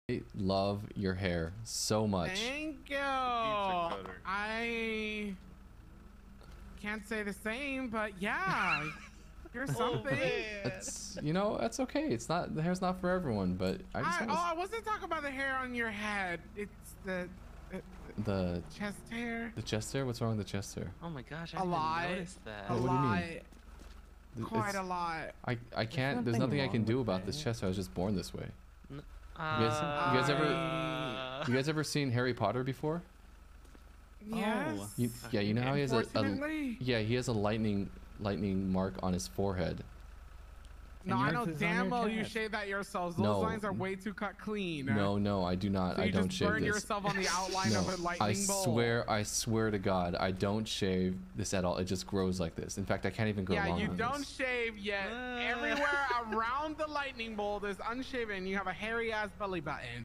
Edison (Michael Hawkins) meets Ricardo for the FIRST time and gets ROASTED
I'm trying to post multiple clips every single day so it helps me more than you think! ❤️